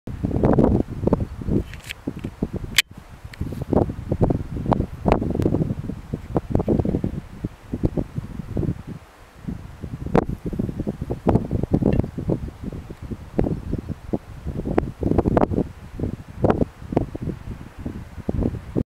bearded vultures.MOV
Gypaètes barbus otherwise known as lammergeiers or bearded vultures near Pic de Séron, Ariège French Pyrénées